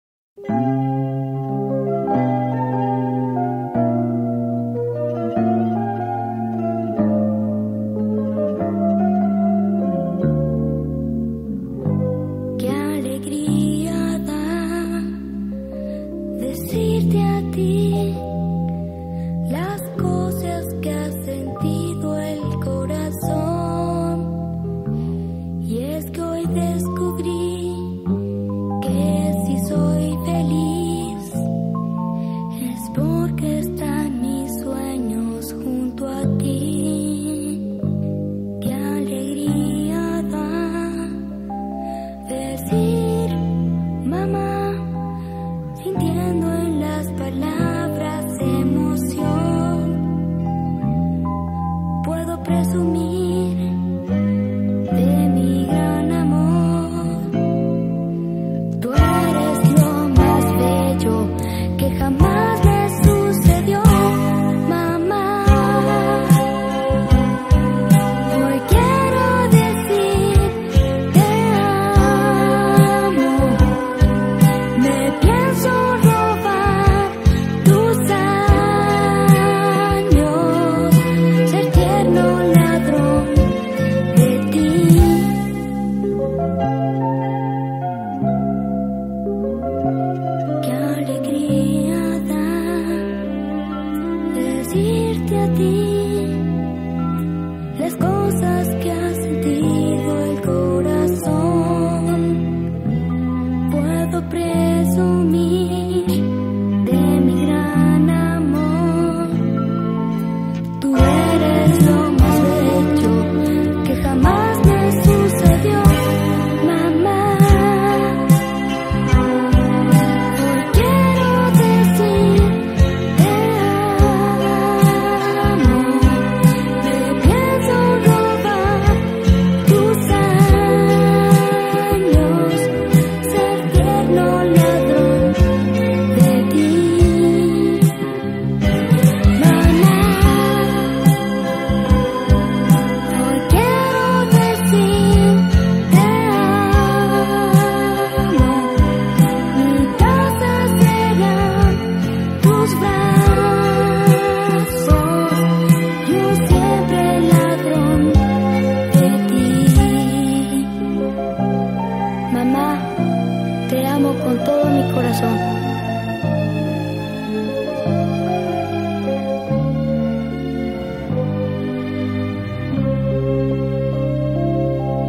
feliz dia 10 de mayo dia de las madres
video dedicado para todas las mamas del facebook y amistades y compañeras de trabajo felicidadez para todas las mamas en su dia